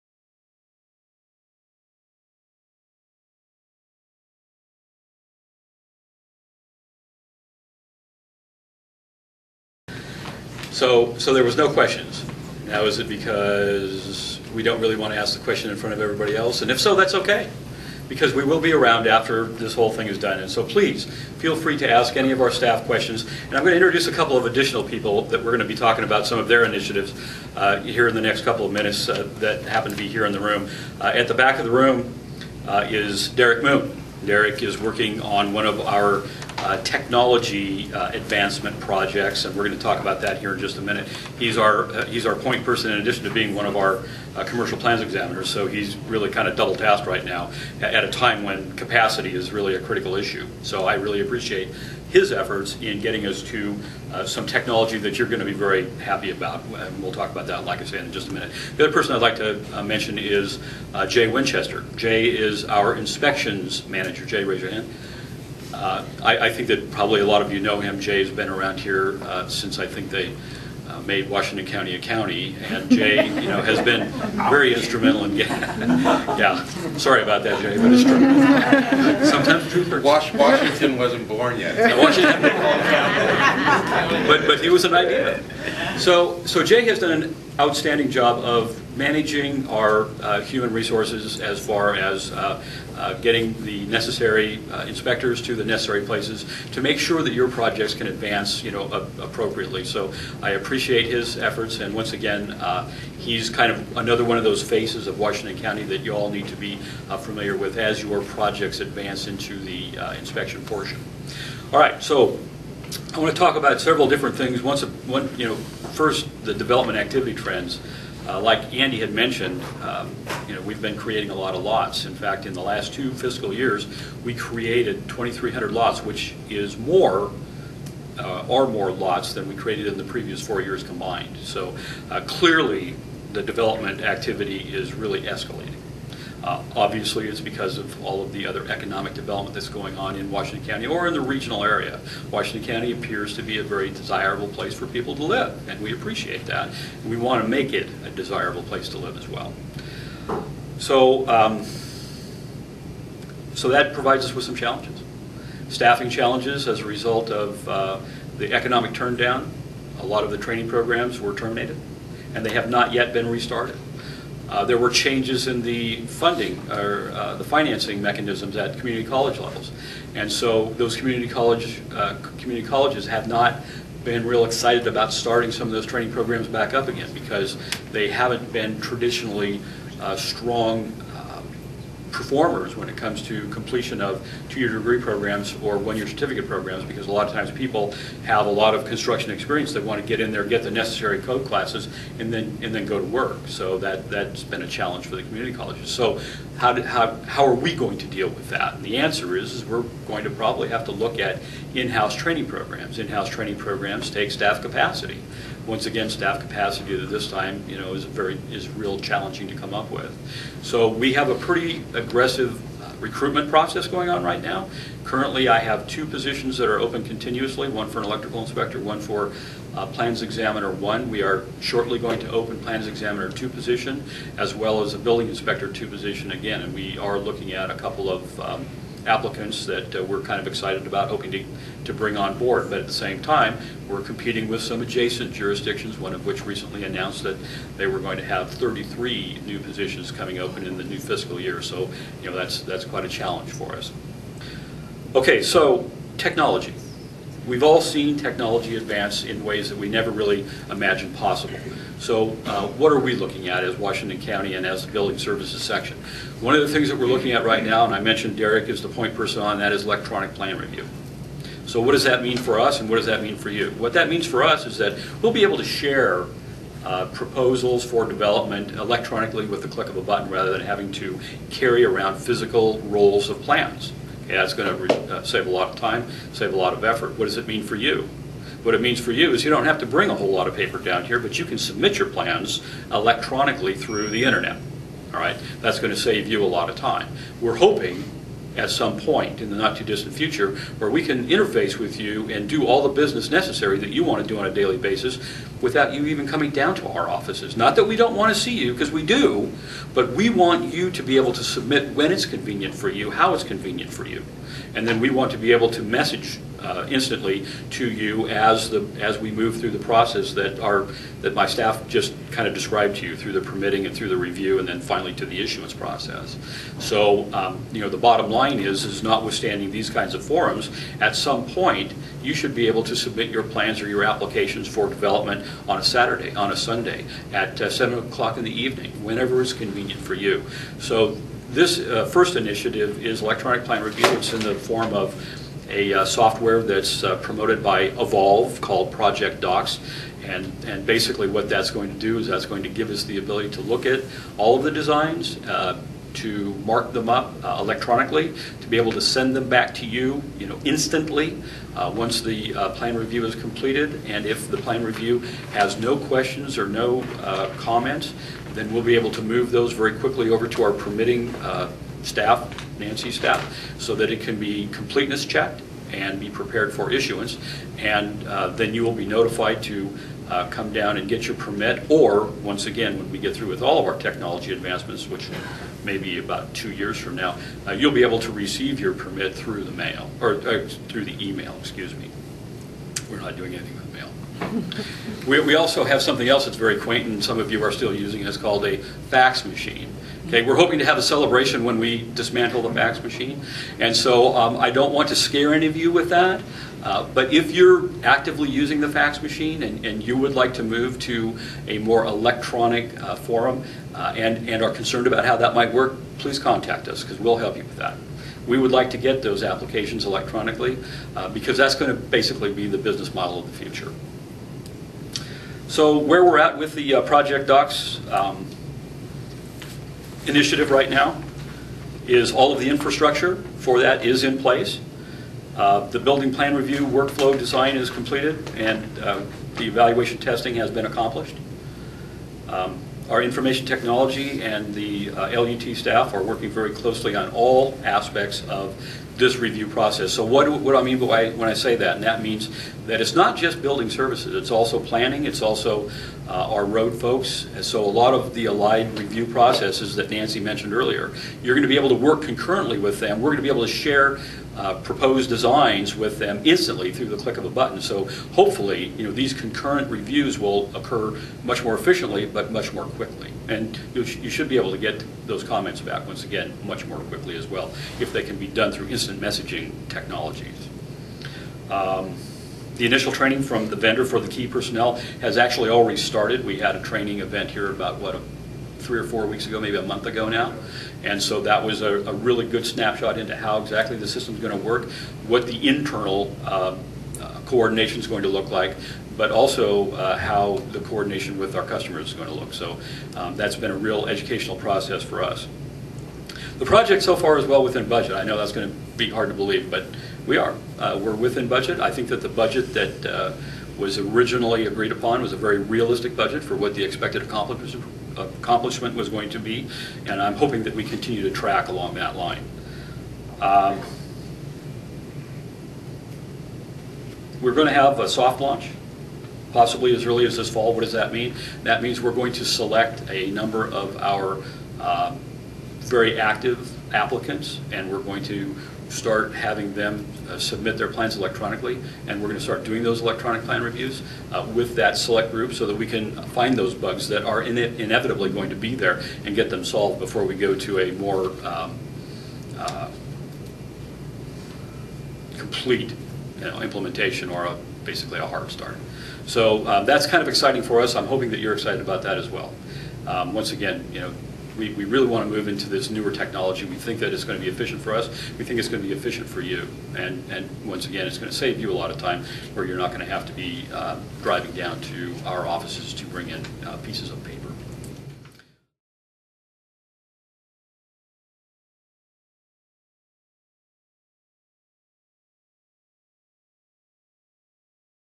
Development Forum April 30, 2015 - Technology Initiatives
Hank McDonald, Building Official
Development activity trends
- Technology Initiatives
o Electronic Plan Review
o Permitting System
o Video Inspection Pilot